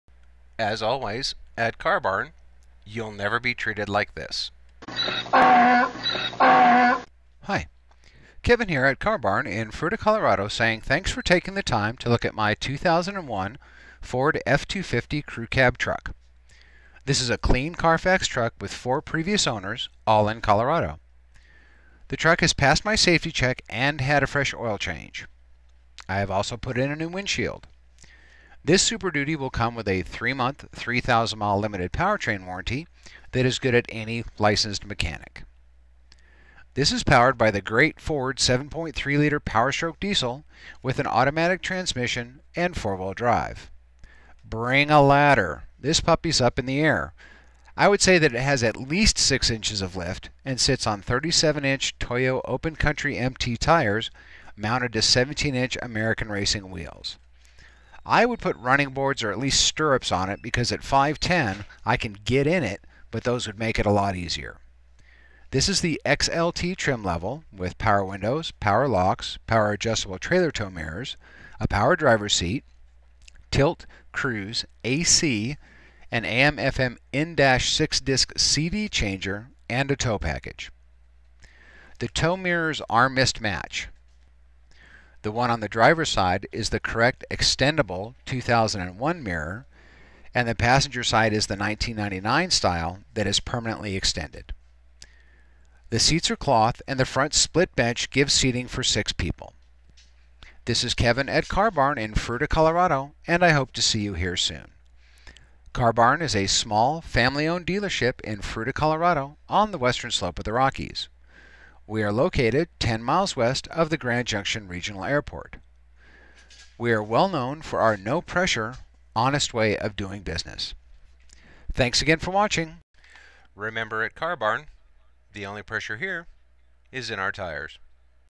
SOLD!-2001 Ford F250 Crew Cab Powerstroke Lifted at Car Barn in Fruita, CO near Grand Junction
Video Walkaround of Stock #10755 my lifted 01 Ford diesel.